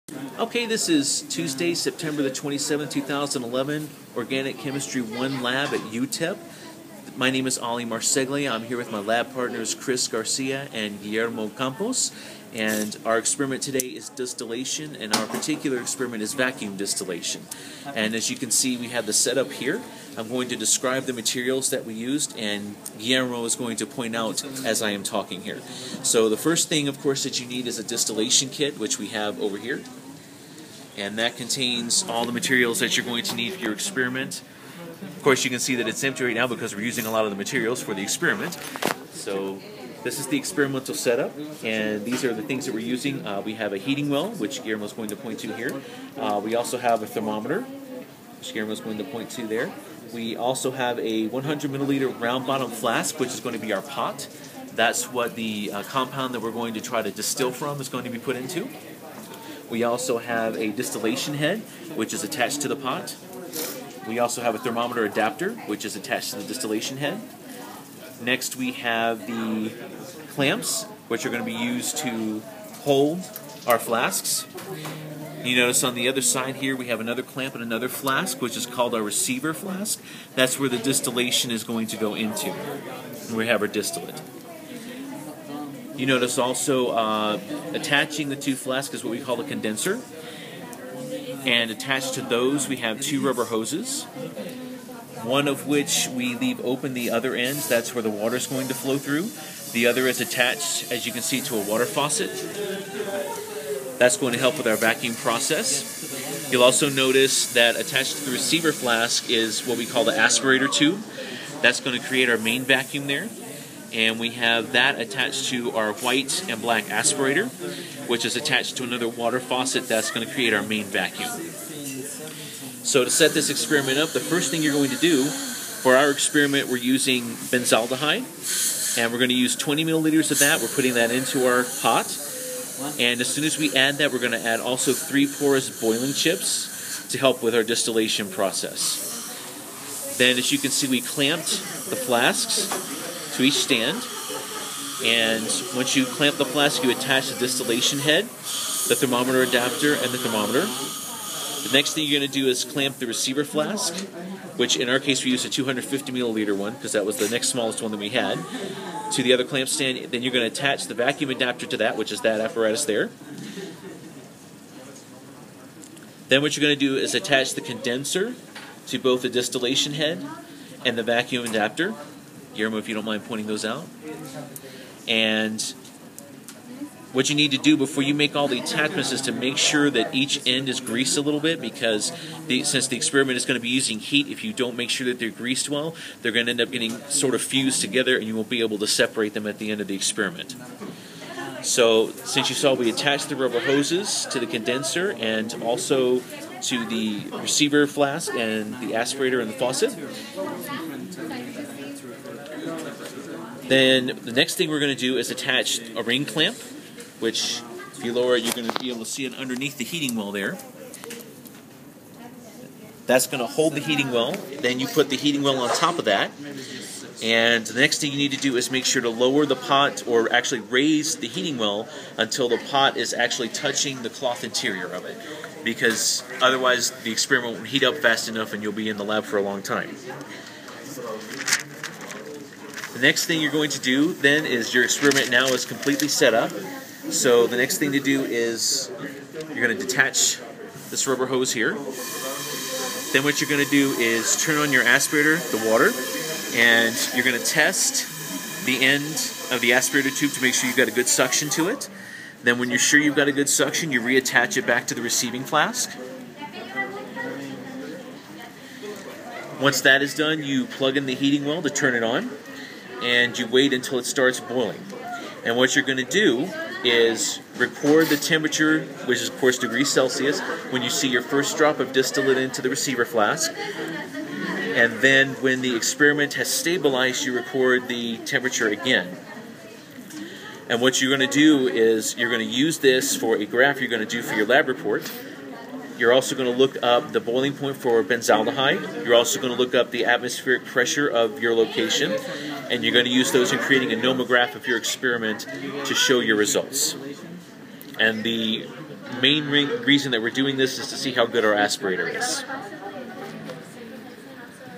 Benzaldehyde Vacuum Distillation Group 4 (UTEP) Set Up
Organic Chemistry 1 Lab September 27, 2011